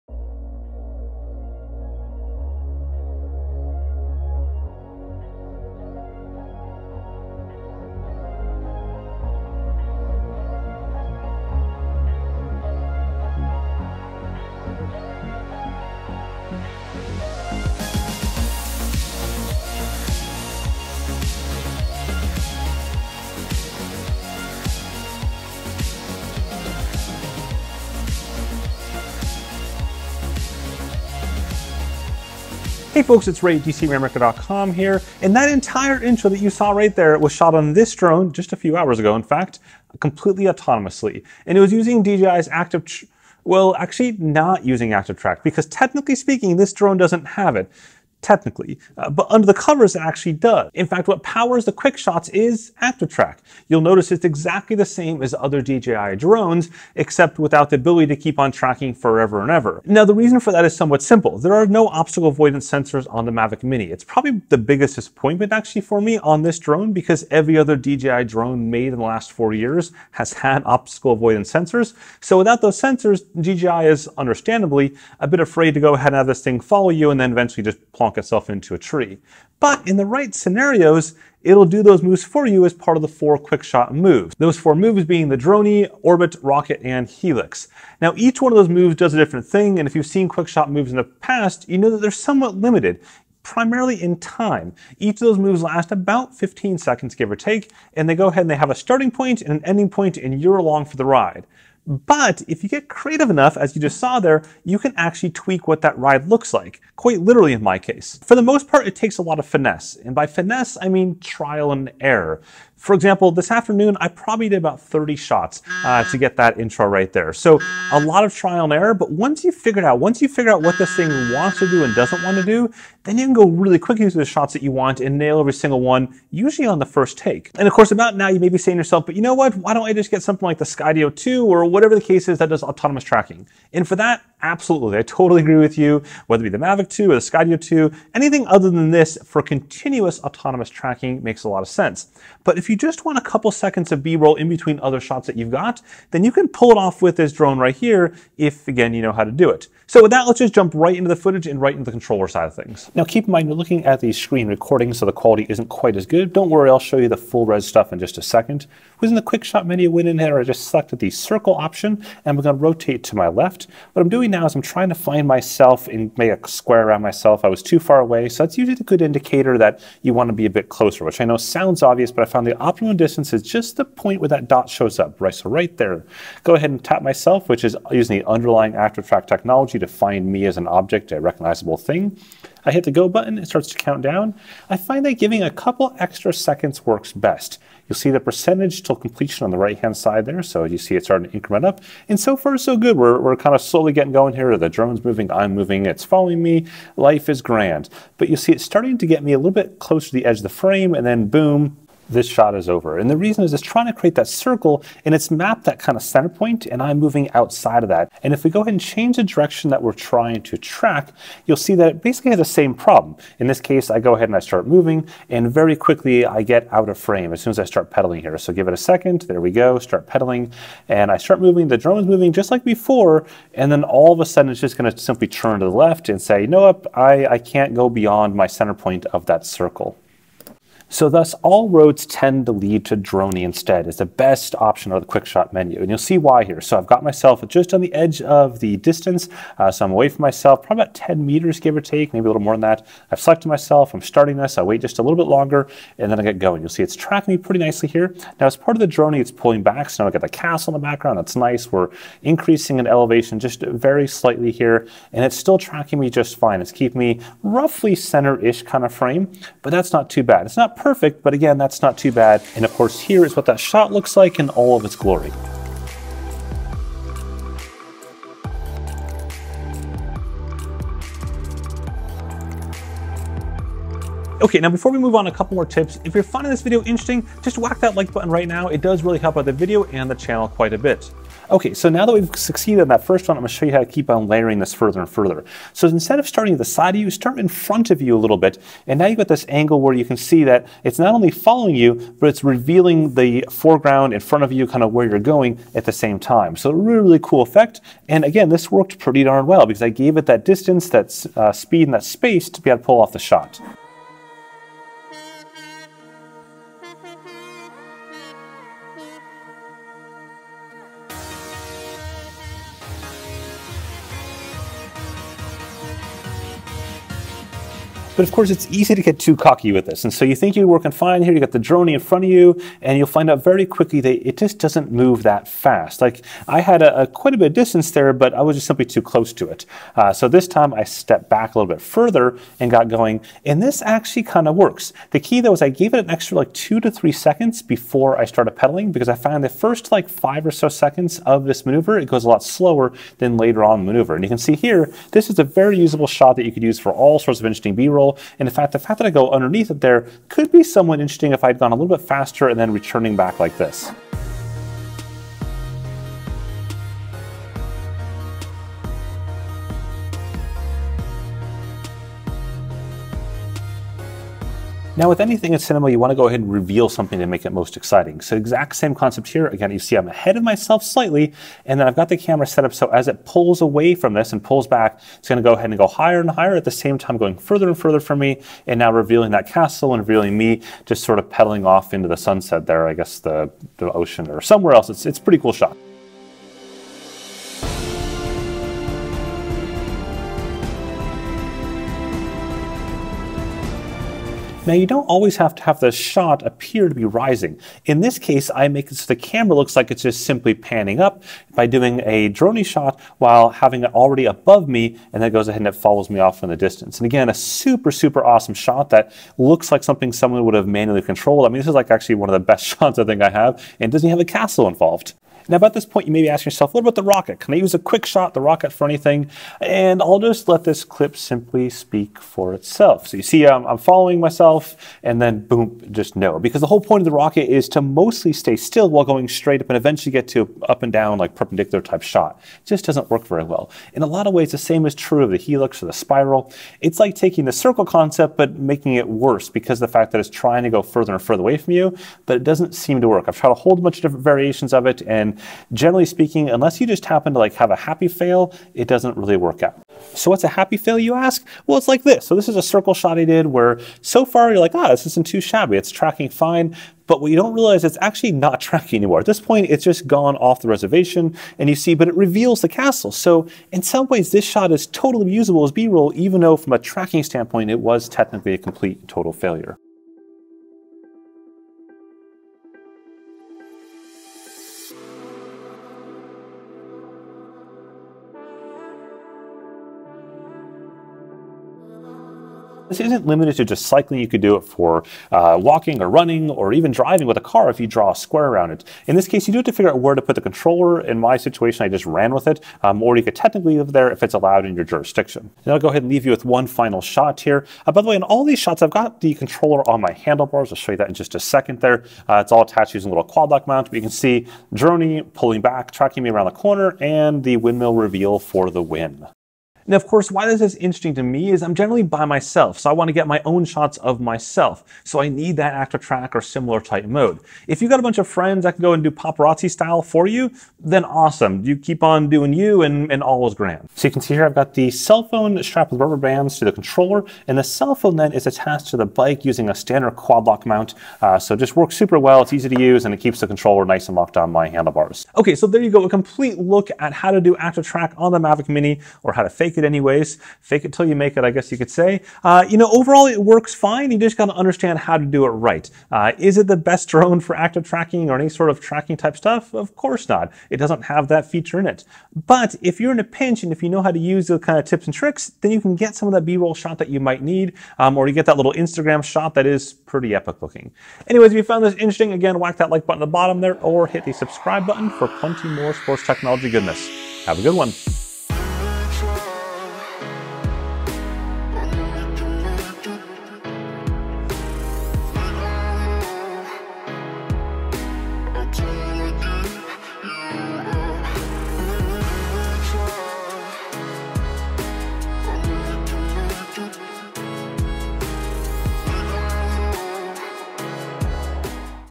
DJI Mavic Mini: How to create ActiveTrack-like sport shots!
DJI Mavic Mini (link helps support channel, thanks!!!)

For real, I'm going to show you how exactly to create incredible autonomous drone tracking shots using the DJI Mavic Mini drone, despite the fact that it technically doesn't have ActiveTrack in it, but rather a dumbed down QuickShot set. Still, with a few tips you'll nail some amazing shots while solo, especially for sports type shots like cycling, running, and more.

I dive into which of the DJI QuickShot modes work well to fake ActiveTrack, and which ones don't. From Dronie to Helix, Rocket to Circle, I give the pros and cons of each, and recommendations on distances and speeds to make incredible b-roll shots.

Main Camera
Action Cam